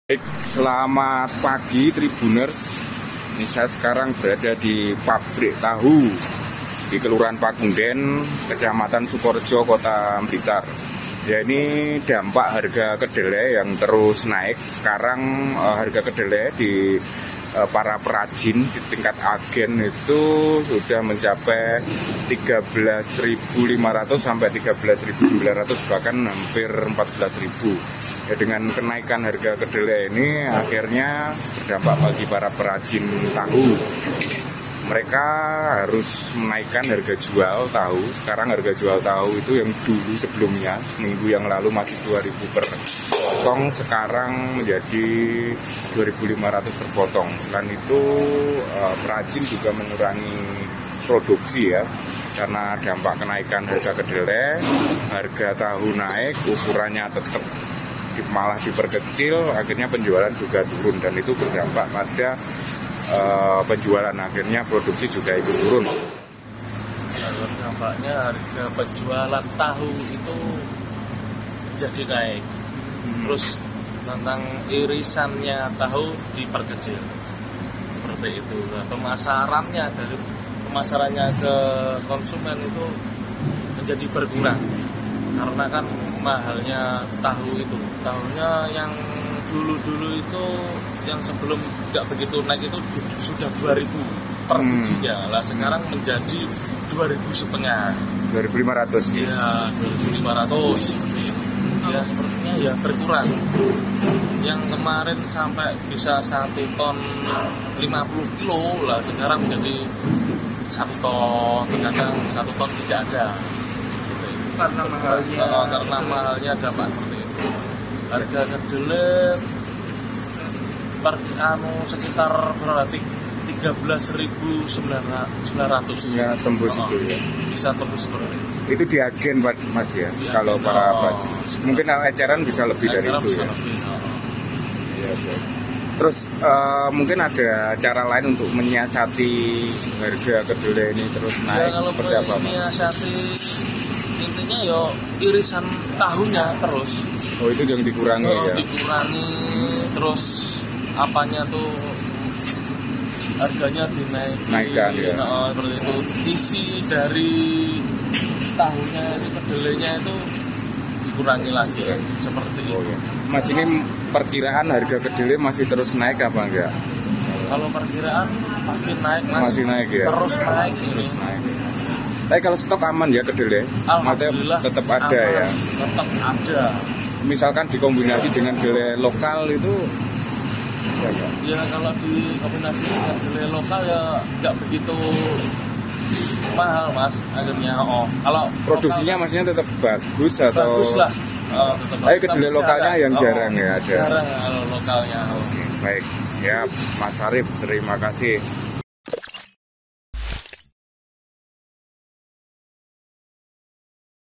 Harga Jual Tahu Naik, Dampak Mahalnya Harga Kedelai
vp : didit budi w / reporter : samsul hadi

TRIBUNMATARAMAN.COM | BLITAR - Harga jual tahu naik dampak mahalnya harga kedelai. Suasana di pabrik tahu Kelurahan Pakunden, Kota Blitar, Selasa (8/11/2022).